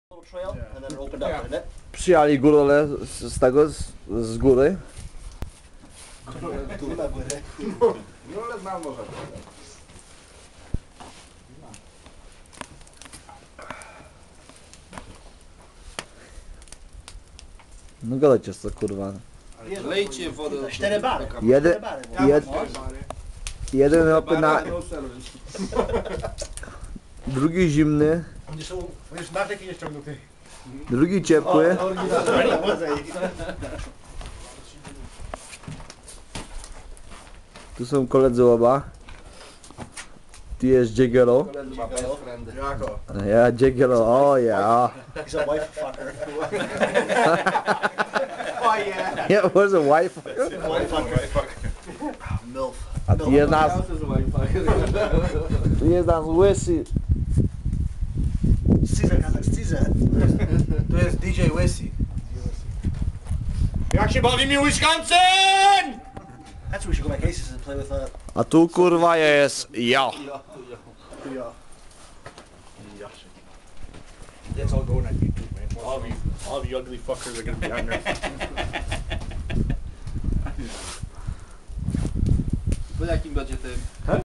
bull shitten at the shrine
us talken just bull shitting and haven a drink